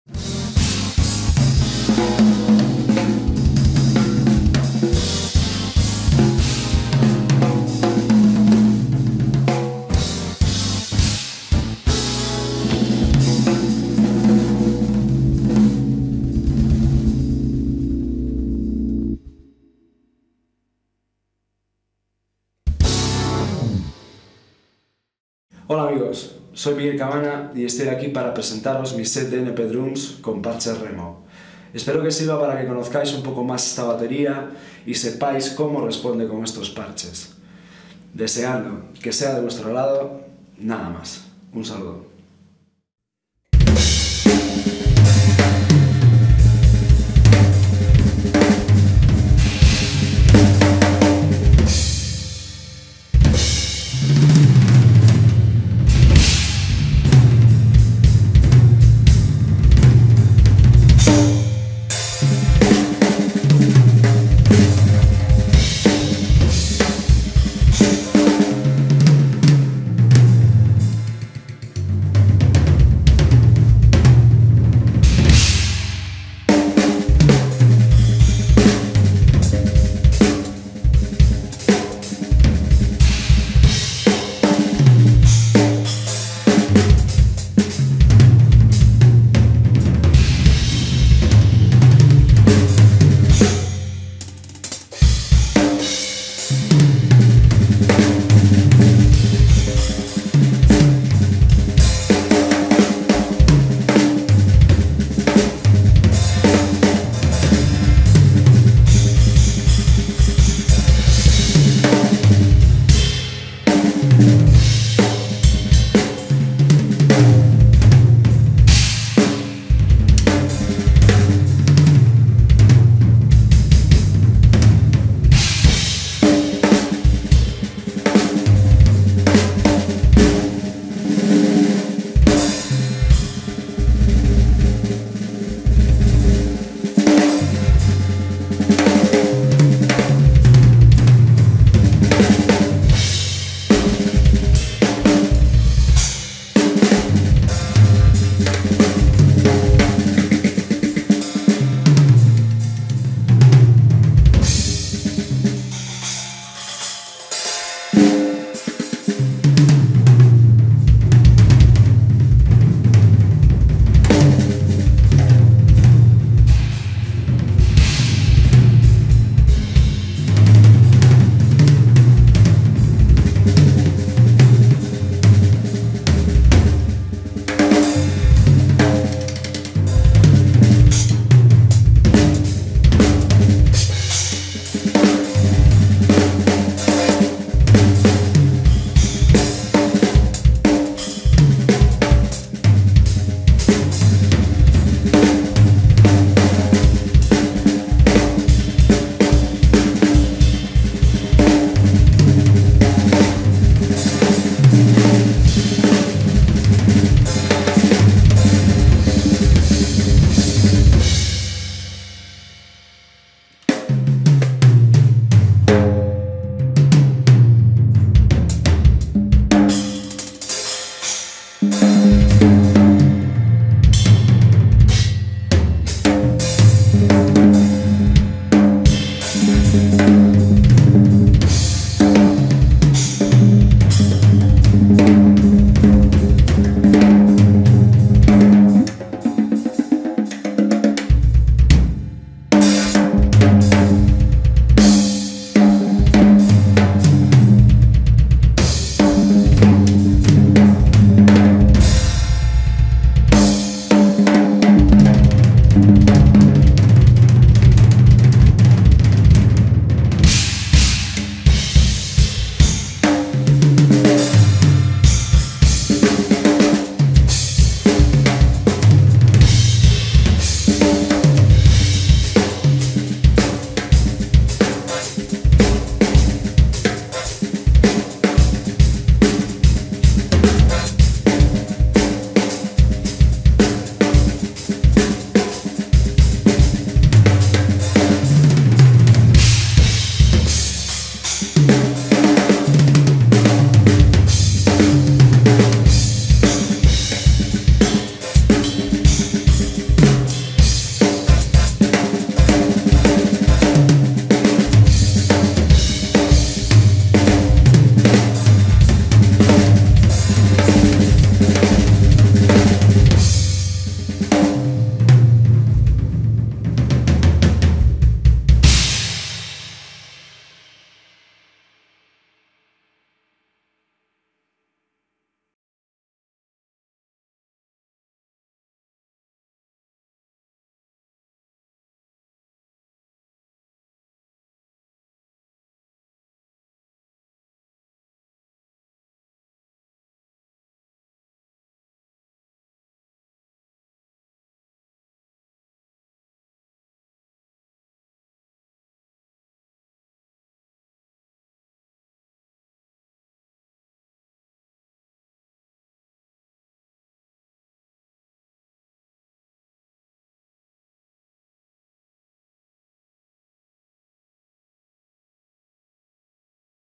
Miguel Cabana con Np drums & Remo drumheads
Vídeo promocional de Np drums & Remo drumheads.
Grabado el 30 de octubre de 2010 en Estudios Visue.
Sonido, Vídeo & Fotografía: Nacho Varela
Diseño: Mónica Pan
Ayudante de Vídeo: Bruno Couceiro
Traducción: Mónica Cartelle Gestal